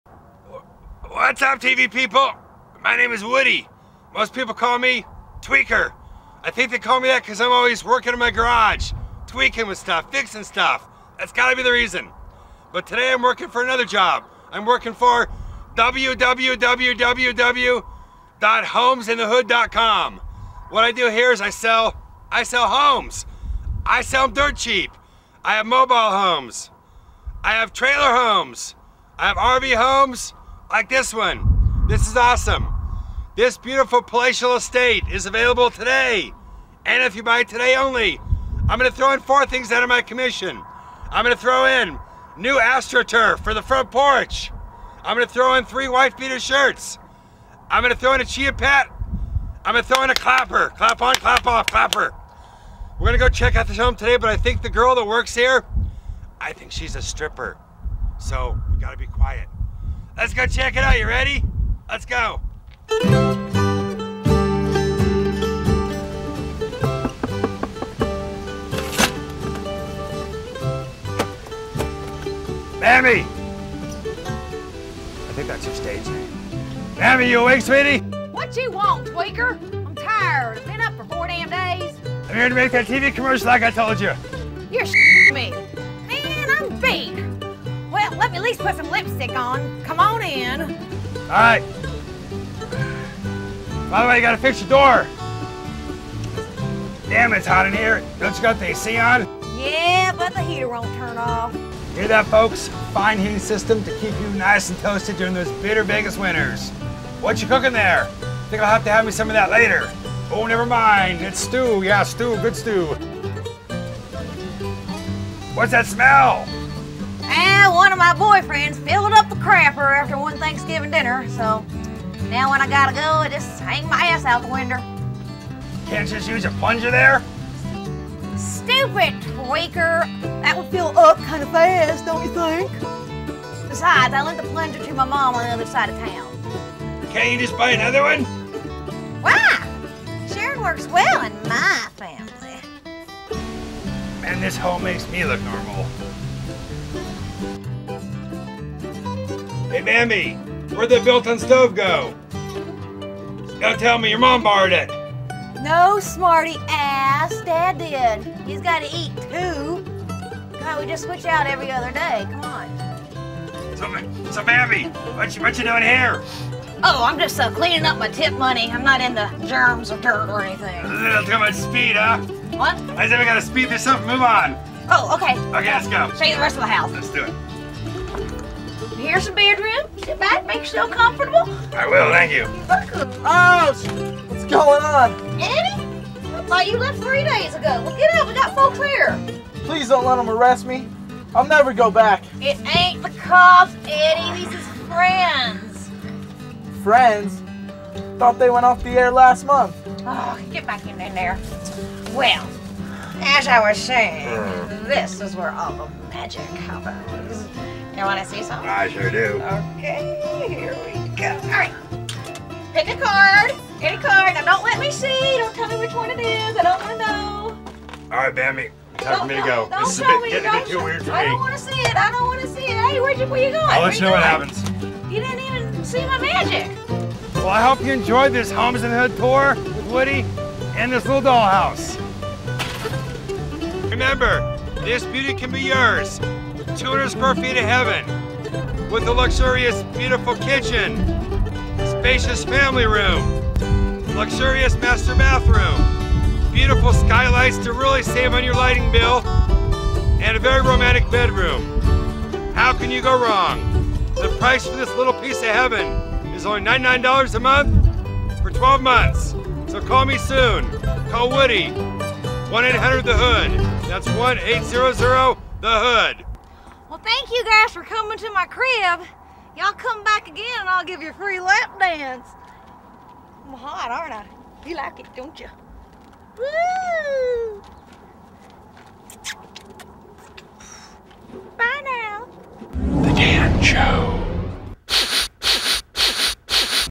Homes in the Hood
A "Realtor" attempts to sell a trailer, owned by a meth cooking stripper.